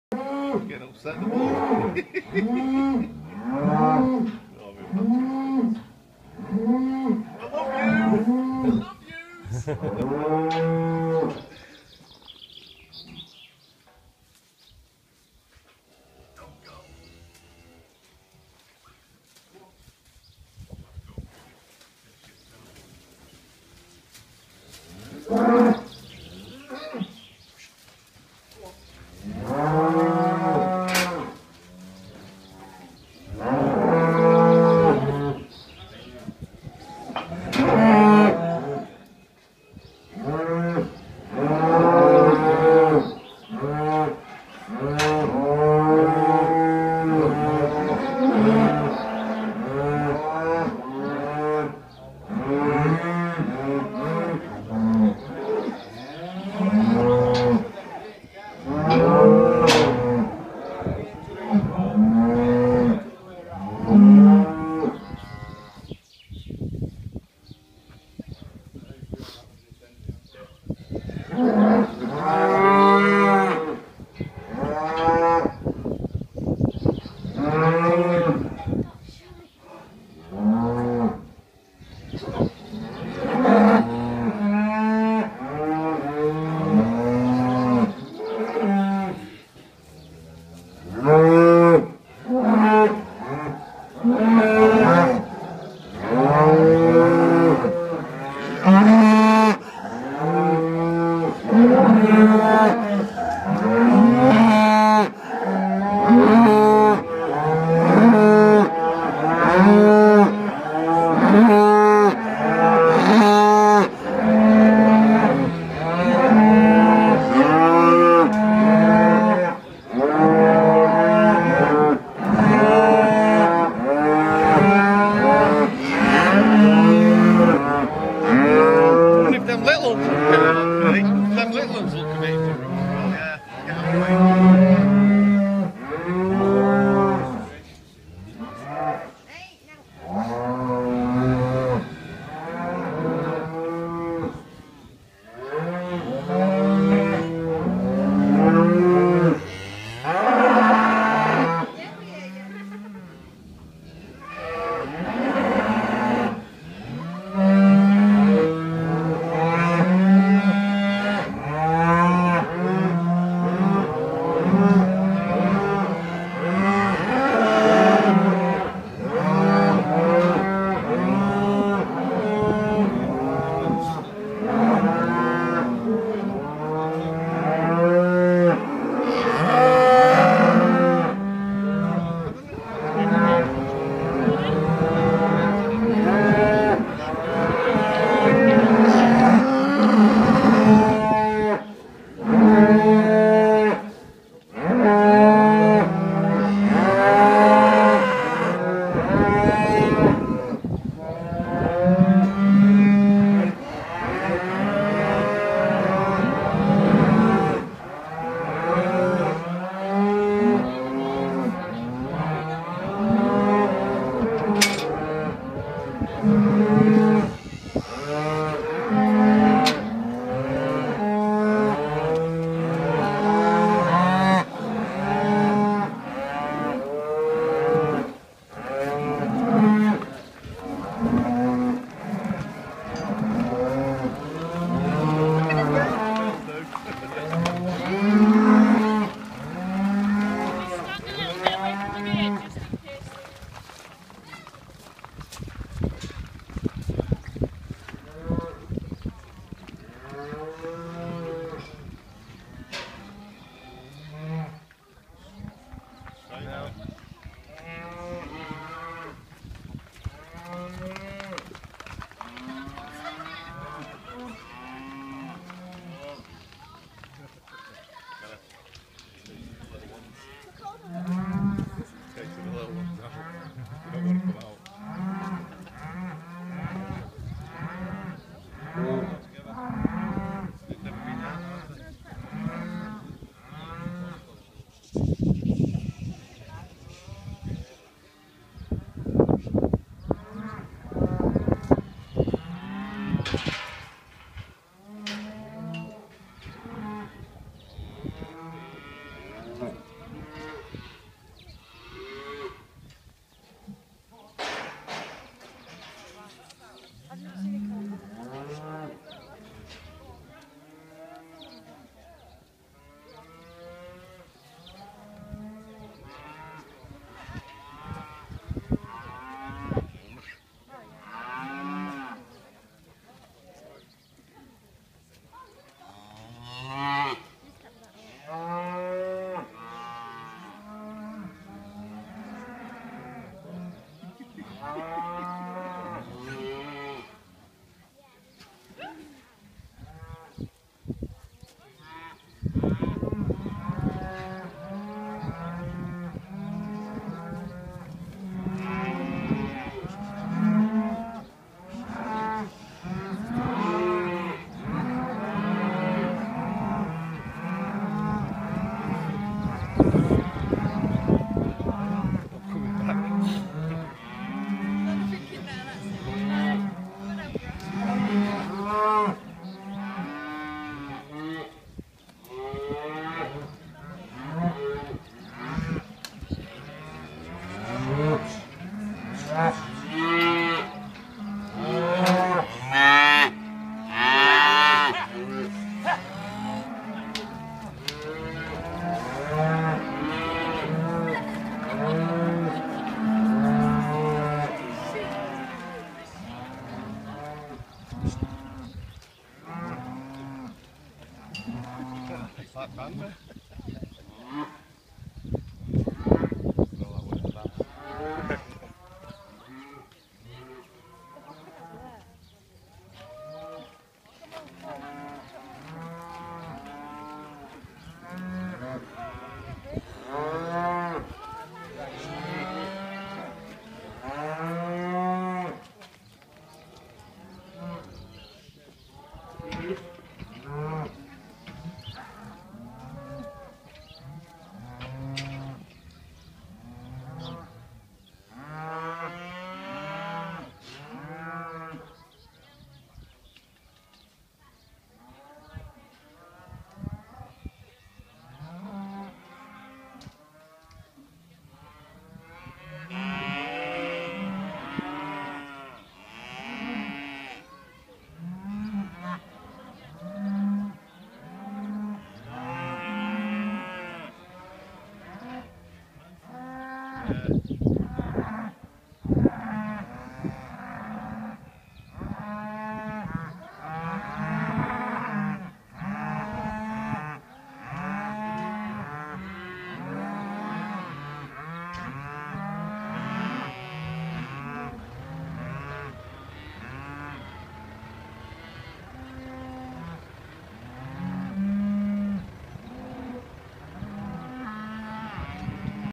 Cows at Dalby
Releasing cows from their winter barn